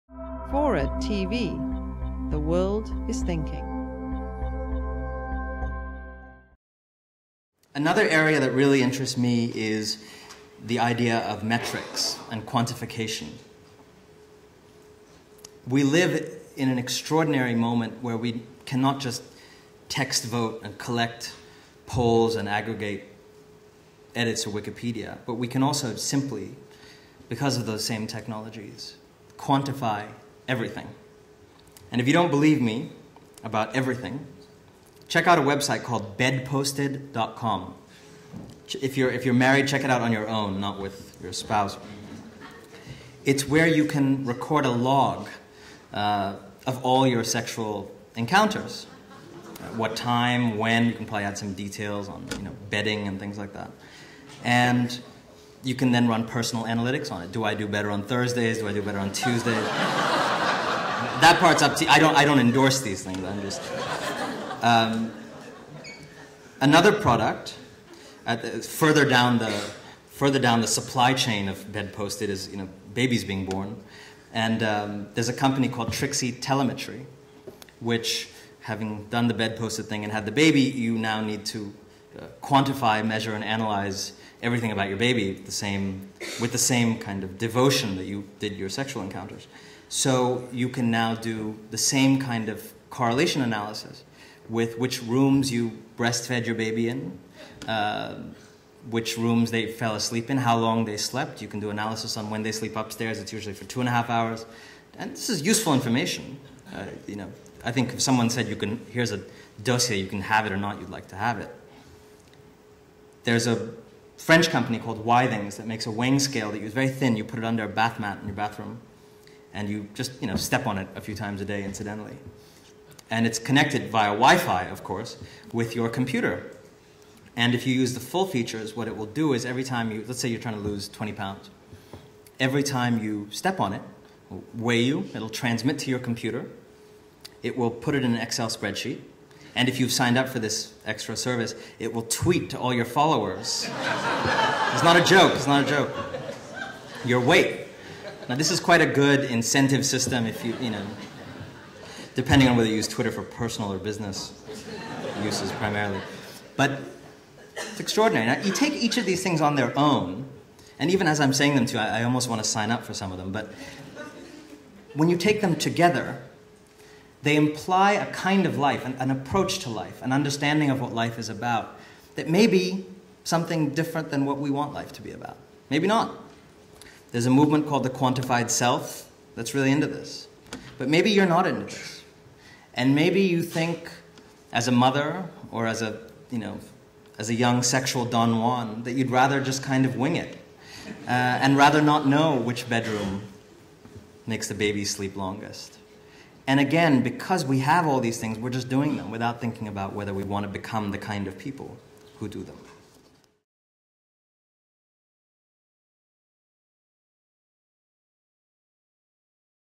A Scale that Tweets Your Weight? Meet the Quantified Self
A scale that automatically tweets your weight? How about a website that analyzes the specifics of your love life? Journalist Anand Giridharadas reflects on burgeoning technologies that facilitate the quantification of daily life, and suggests users consider the deeper implications of the "quantified self."

As social networks become more pervasive and change the way we interact, New York Times  columnist Anand Giridharadas asks what happens "to our minds and our hearts when we become digital people?" Is the social web the world finds itself tangled in a totally new experience?

Anand Giridharadas doesn't think so. He draws parallels between the "ambient sociability" of village life in Bombay with the sprawling online communities we now interact with daily. - Australian Broadcasting Corporation

Anand Giridharadas is a writer based in Cambridge, Massachusetts. His first book, a work of narrative nonfiction about his return to the India that his parents left, is forthcoming from Times Books in early 2011. It is titled India Calling: An Intimate Portrait of a Nation's Remaking.

He is a columnist for The New York Times and its global edition, the International Herald Tribune: his Currents column explores fresh ideas, global culture and the social meaning of technology. In 2009, he completed a four-and-a-half-year tour as a foreign correspondent in India for The Times and the Herald Tribune, as their first Bombay presence in the modern era. He reported on India's transformation, Bollywood, corporate takeovers, terrorism, outsourcing, poverty and democracy. He was appointed a columnist in 2008, writing the Letter from India series.